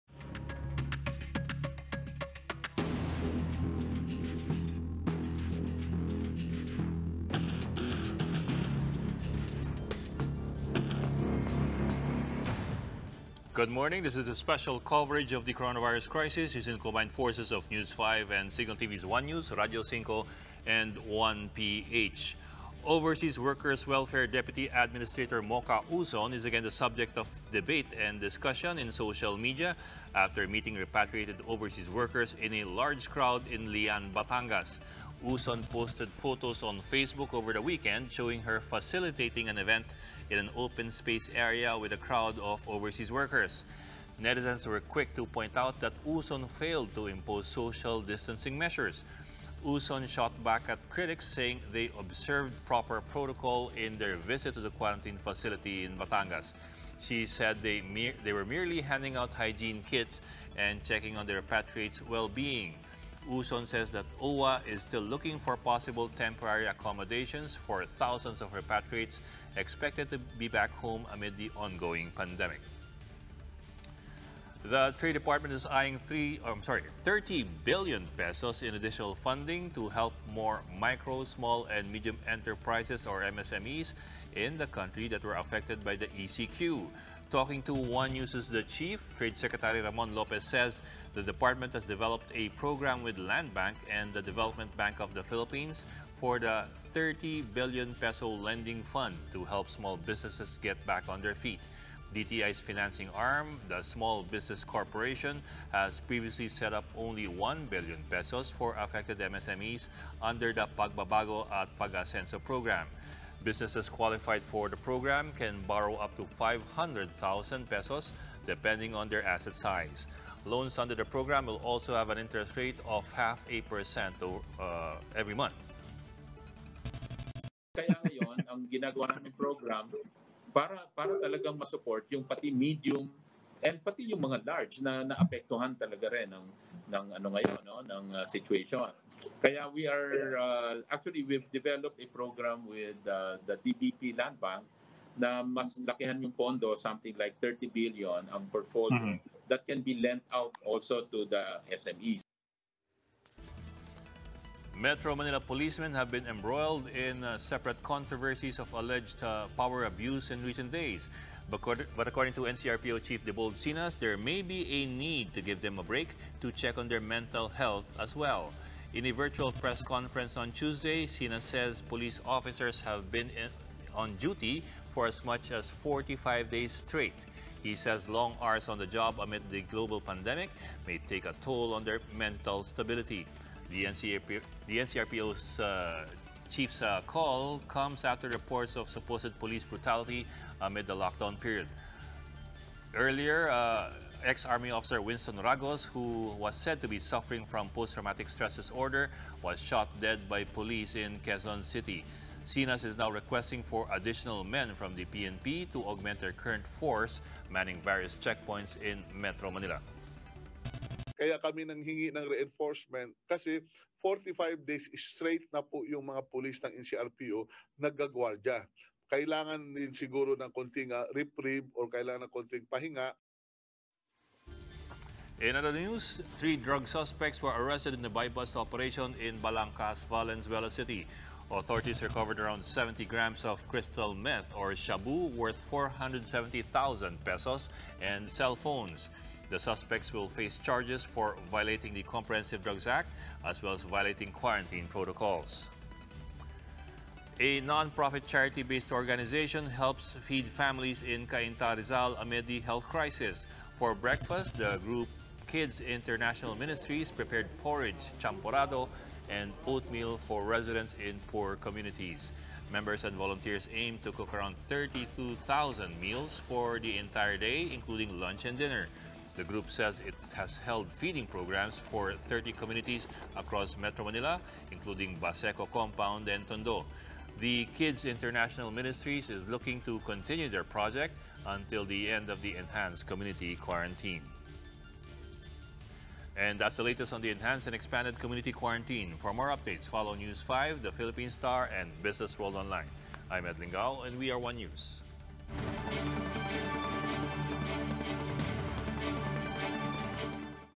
ONE NEWS NOW | April 29, 2020 930am
• OWWA deputy administrator Mocha Uson draws flak over large event in Batangas
• DTI eyes P30B upgrade in loan assistance for MSMEs
• NCRPO seeks mental health break for personnel
• Three arrested in buy-bust operation in Valenzuela City
• Charity feeding program held for poor communities in Cainta, Rizal